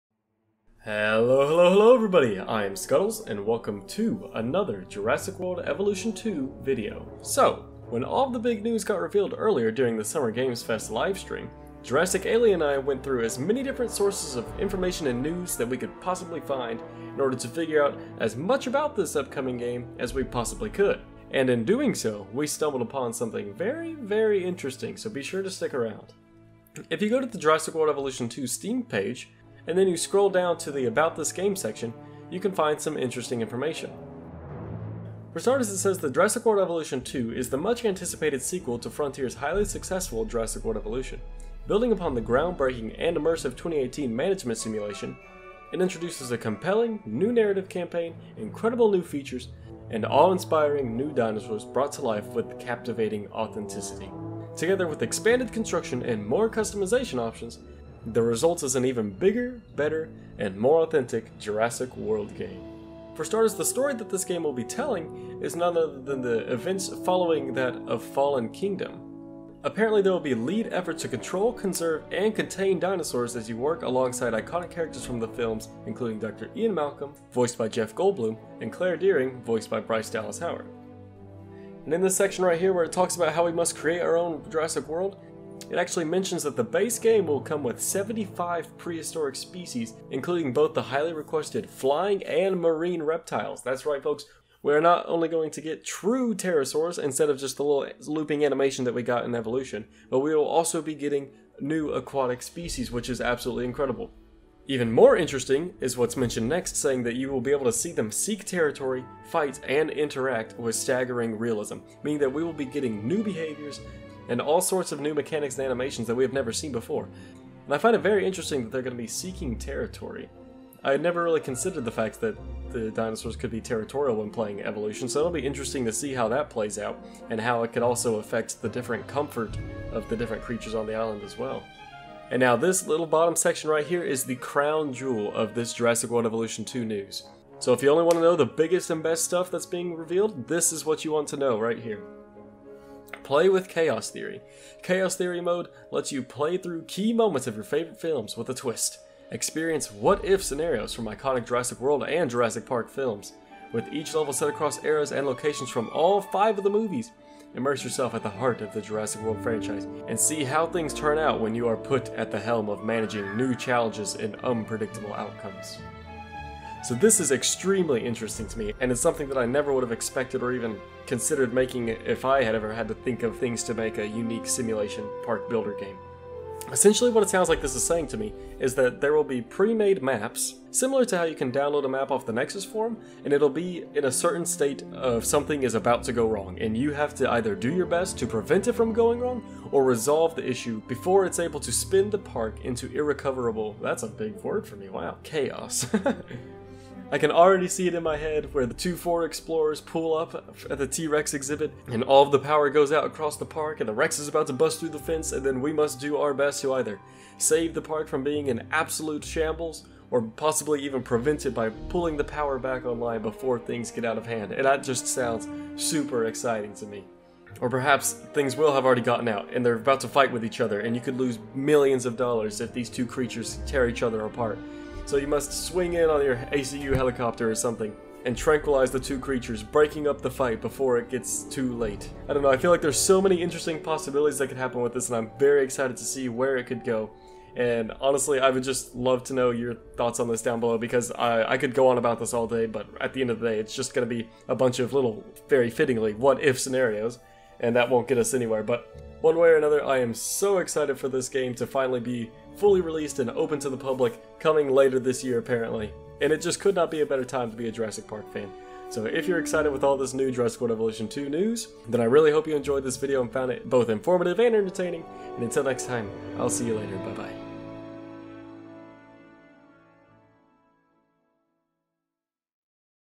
*NEW* CHAOS MODE For Jurassic World Evolution 2!
Here we dig into the notes from the Official Steam Page for Jurassic World Evolution. After investigating for juicy secrets within these notes we managed to discover some exciting new mechanics, features, and more that will be making an appearance when Jurassic World Evolution 2 is released, later this year.

Are you excited about this news?! If you noticed something I didn't be sure to point it out in the comments down below and perhaps we will make another video on that in the near future!

Jurassic World Evolution 2 Has Officially been confirmed during the Summer Games Event Livestream! Coming later this year (2021) Jurassic World Evolution 2 will be making its big debut sooner than later!